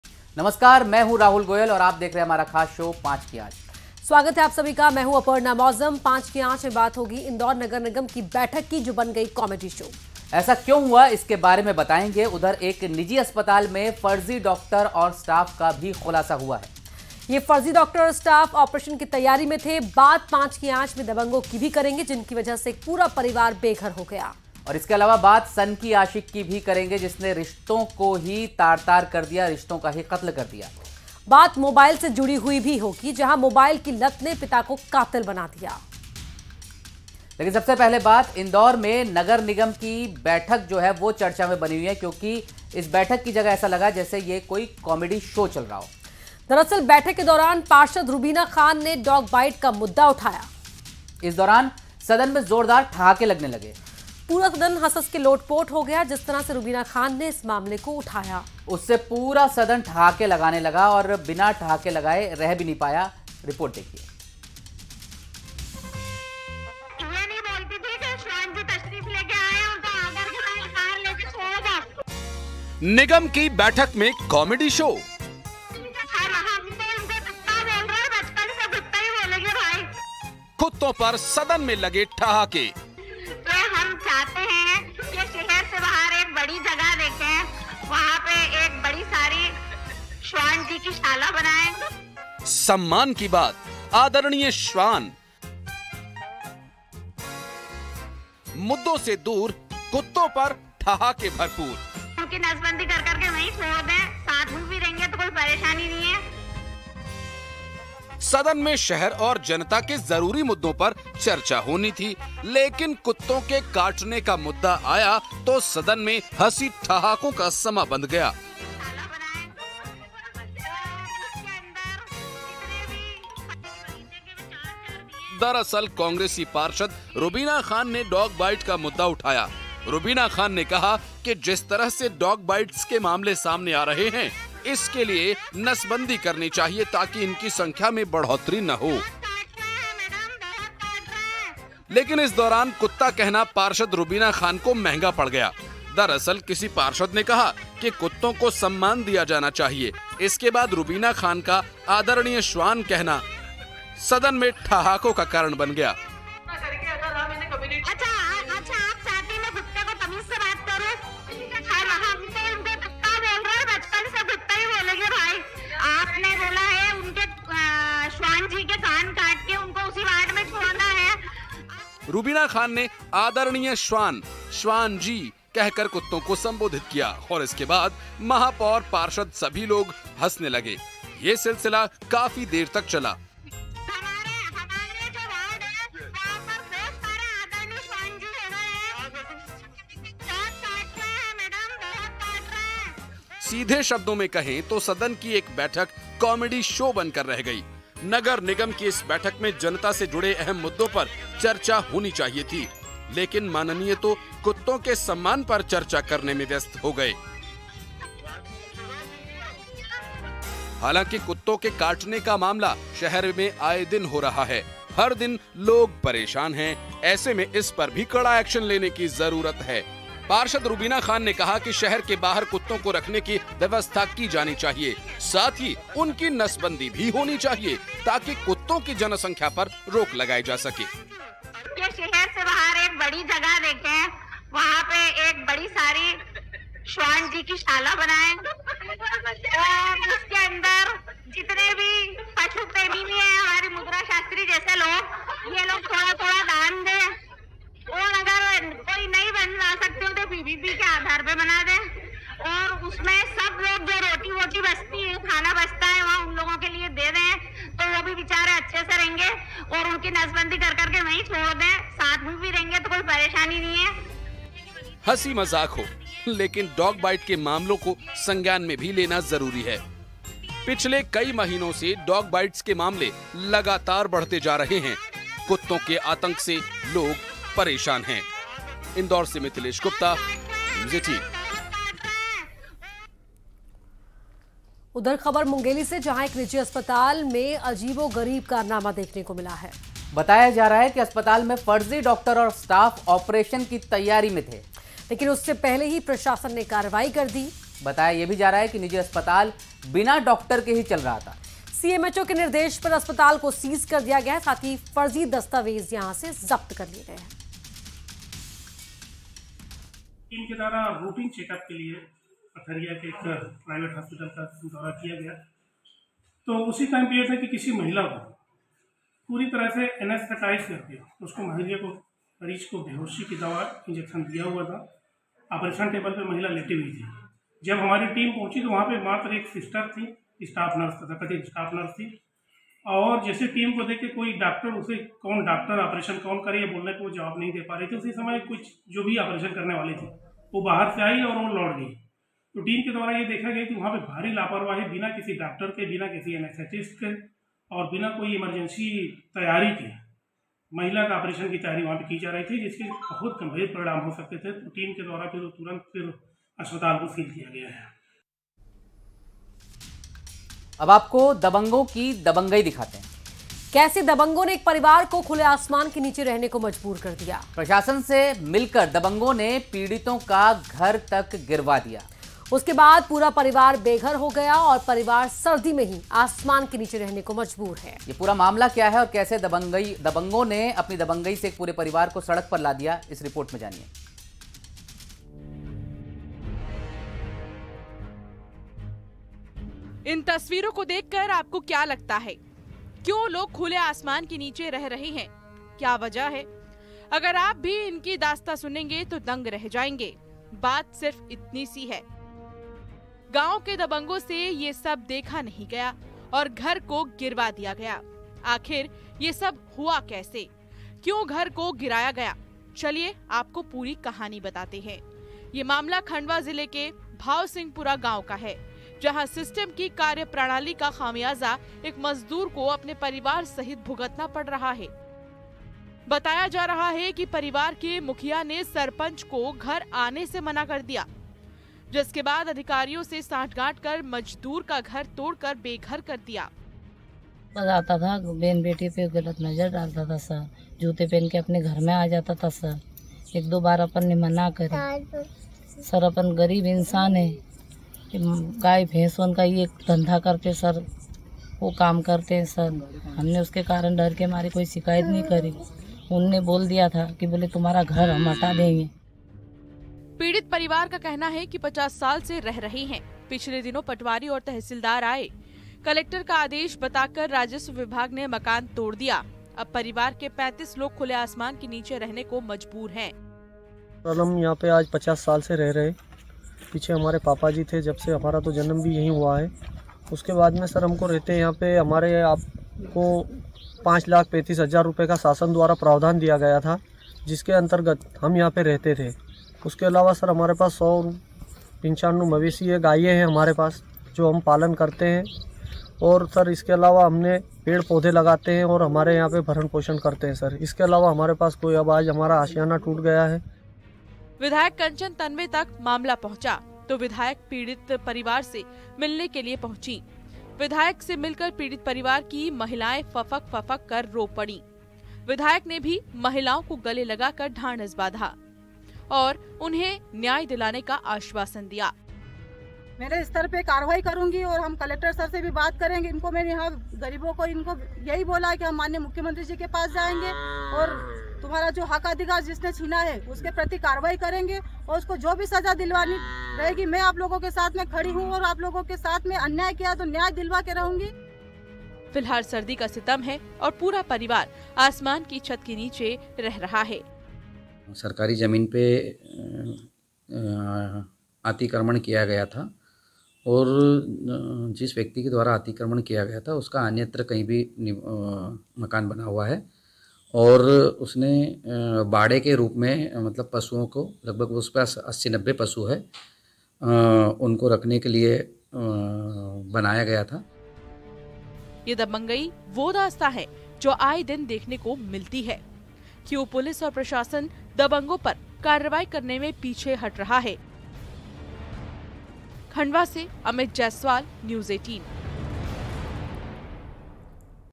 MP News : Indore में Nagar Nigam के सदन में कुत्तों पर जमकर लगे ठहाके! | Breaking News | Rubina Khan
Indore में Nagar Nigam के सदन में कुत्तों पर जमकर लगे ठहाके! रूबिना खान ने कुत्तोंं पर की ऐसी बातें.